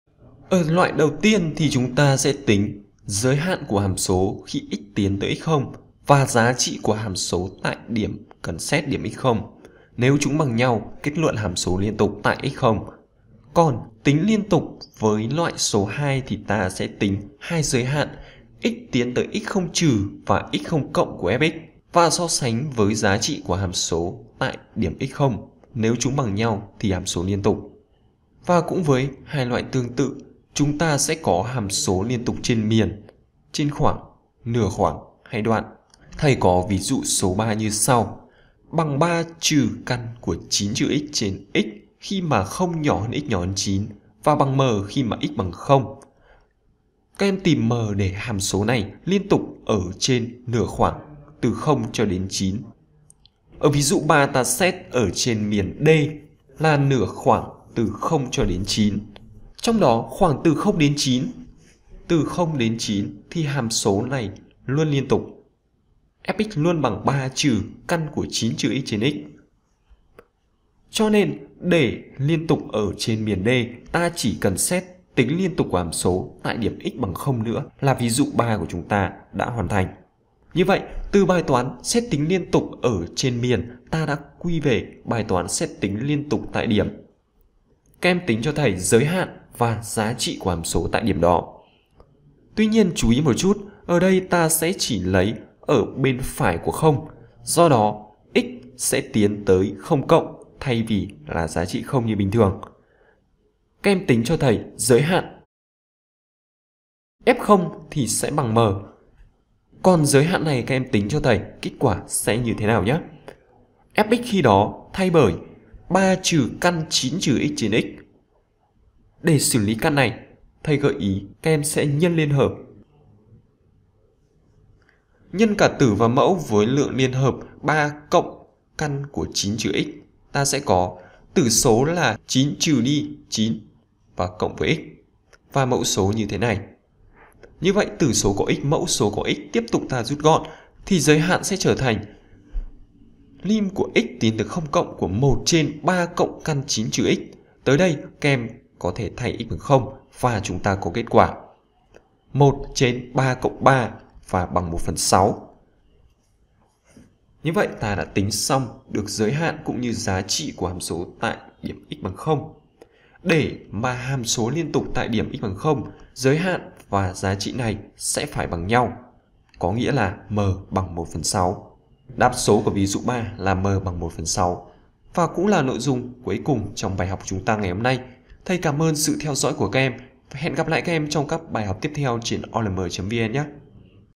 Chuyên đề Hàm số liên tục (Trên miền - Phần 2) - Toán lớp 11 - OLM.VN
Nội dung bài học: Cách xét tính liên tục của hàm số tại điểm và trên các khoảng, đoạn.